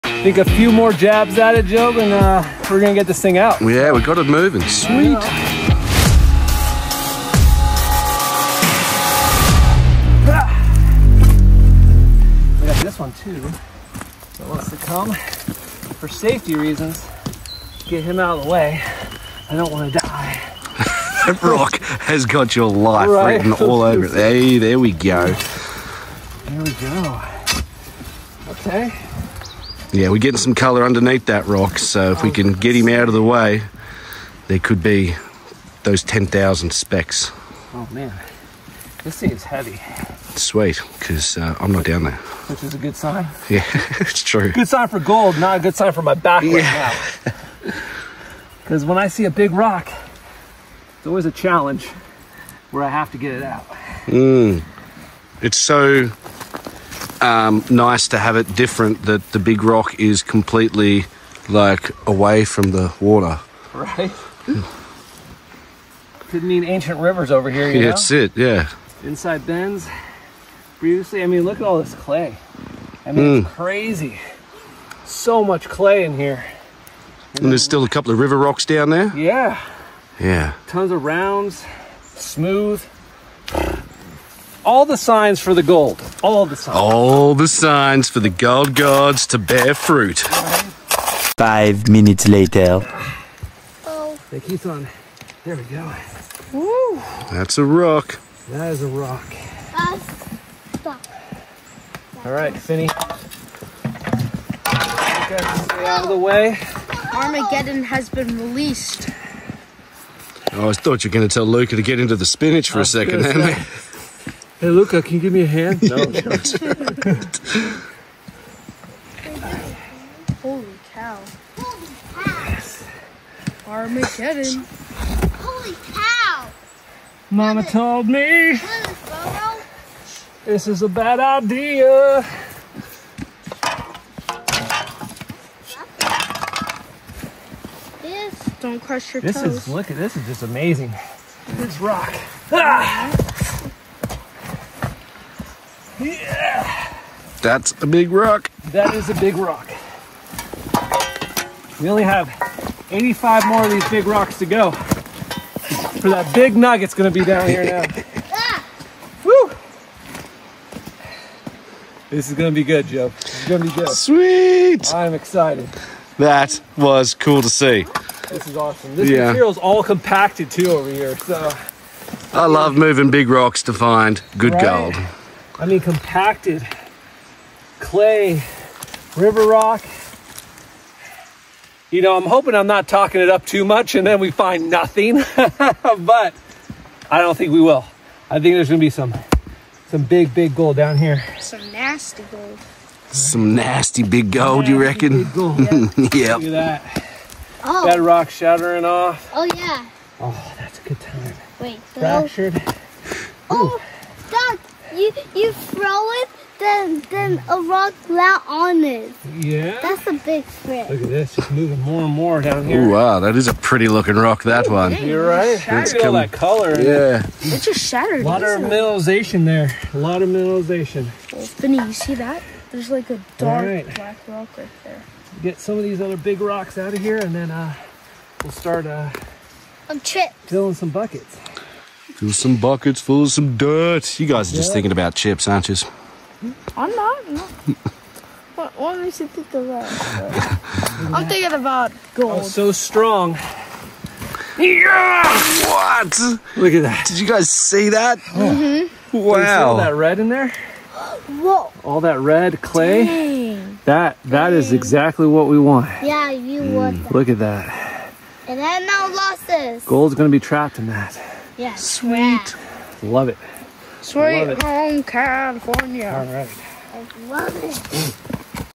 Moving Mountains for Gold: A Behind-the-Scenes Look
Discover the thrill of gold mining in Australia! ⛏️

Join us on an epic adventure as we tackle the daunting task of moving a massive boulder in search of hidden treasure. With my two sons and my trusty friend Jhob by my side, we'll navigate the rugged Australian outback, use our muscles and sheer determination to uncover the gold that lies beneath.

Watch as we:

Explore the remote goldfields of Australia.
Utilize our muscles to move the boulder.
Unearth hidden secrets of the Australian mining history.
Experience the camaraderie and excitement of a true mining expedition.

00:00 Introduction
00:12 The ROCK
01:39 Moving the ROCK
02:39 The ROCK has been REMOVED